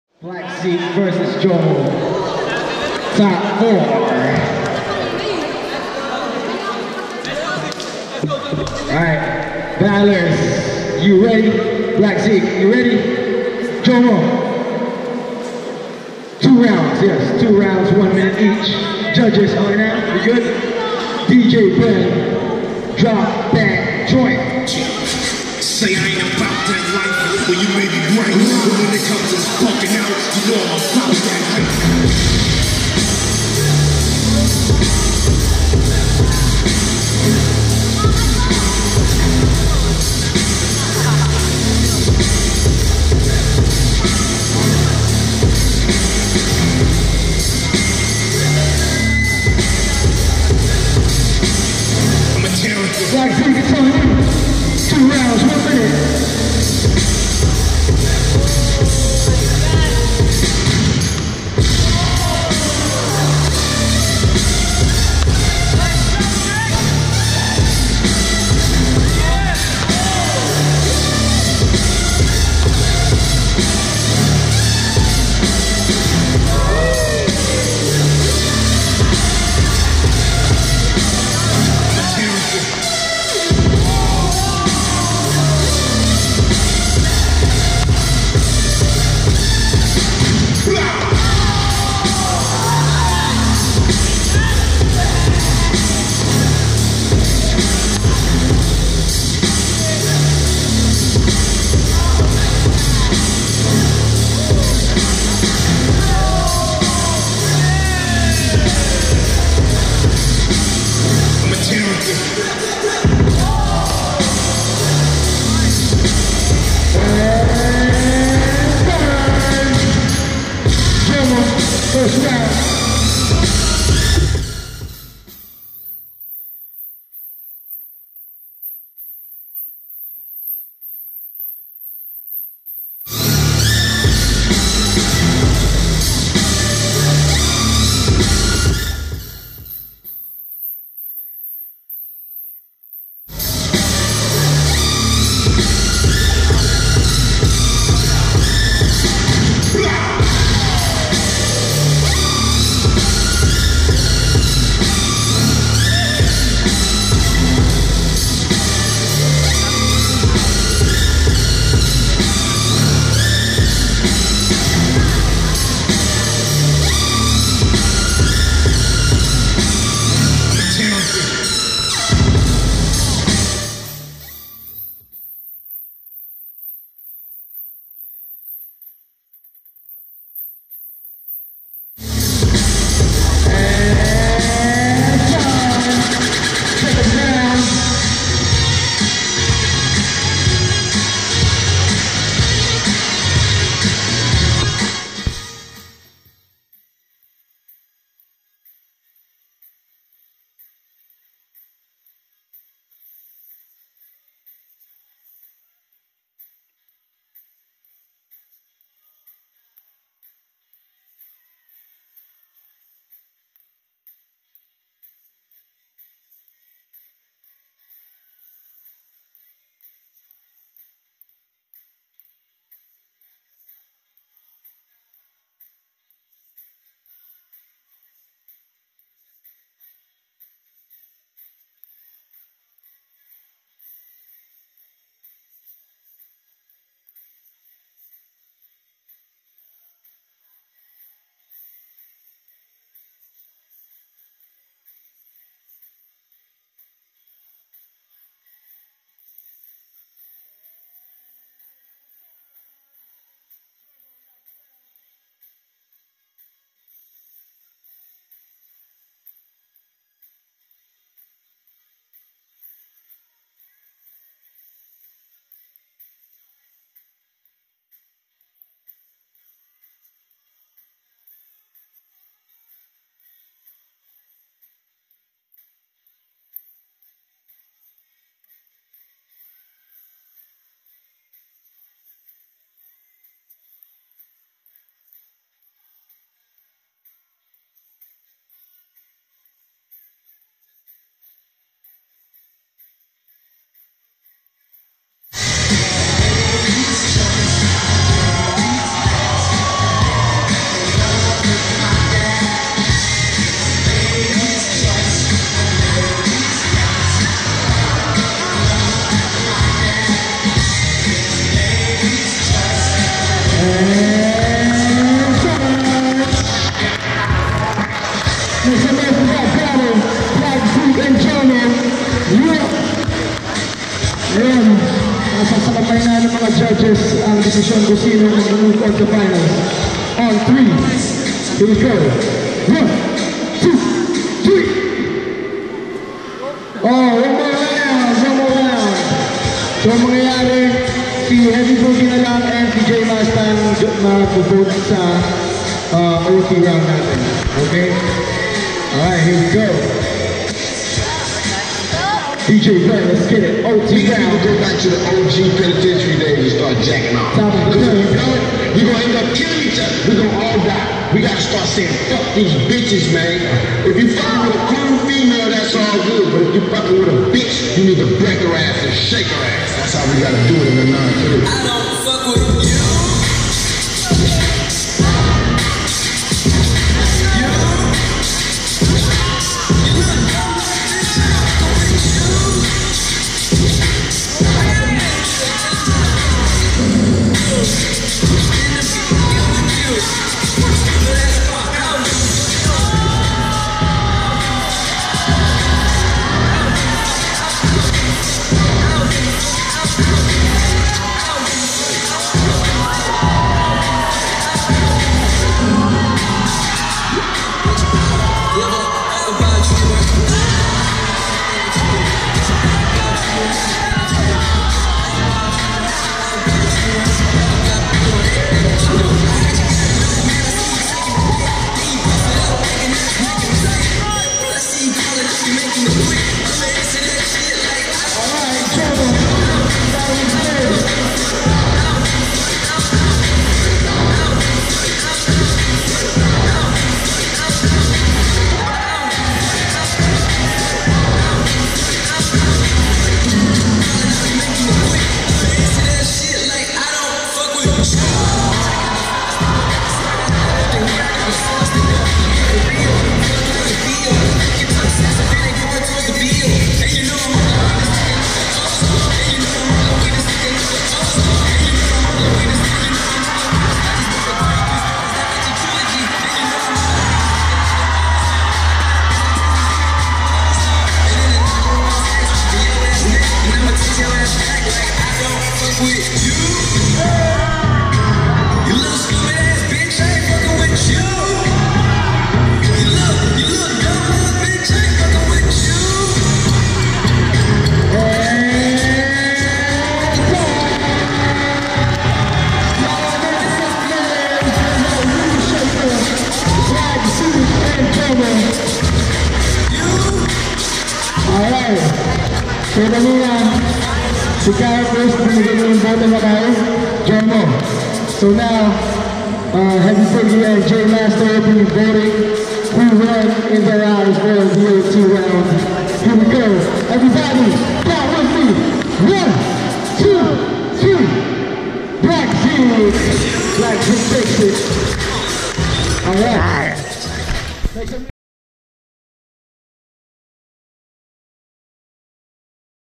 [DFHManila2017] Open Fire Top 4 - Black Zik vs Jomo
Note: Apologies for the muted audio in the middle of the battle. Technical difficulties, apparently.

Vibe Dance Studio
Taiwan Kinetic Art Association

presents

THROWDOWN VOL. 4: Dance For High Manila 2017
Date : 2 July 2017
Time : 1PM - 9PM
Venue : Ayala Malls Vertis North

DJ: Ben Colarina
MC: Switch Sonza

JUDGES
- Funky Luke
- HeavyBoogie
- Jmasta
- Karaquist

Online Media Partner: FilipinoDancers
Photo: Just Jazz Photography
Video: VDS Prod / SPLgum

Supported by:
VLNVA
GaiaRetreatsPH